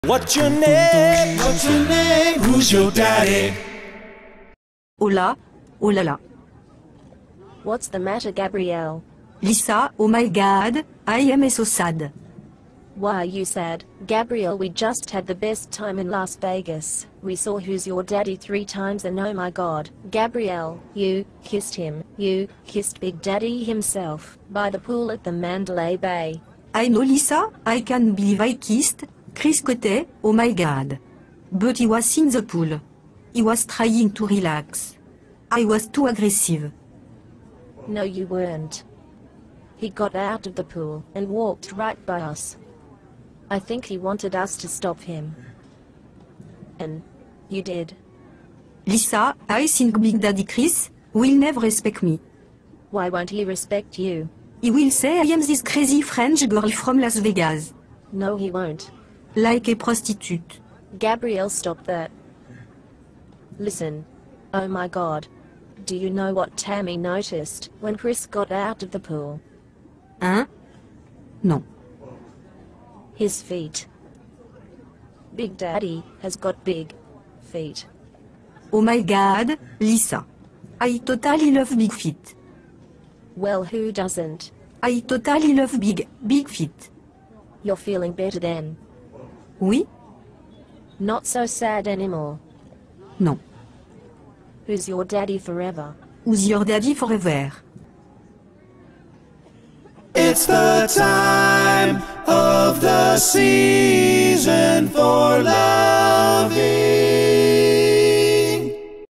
Daddy's Girls - French Kiss
A Big Daddy encounter by the pool in Vegas.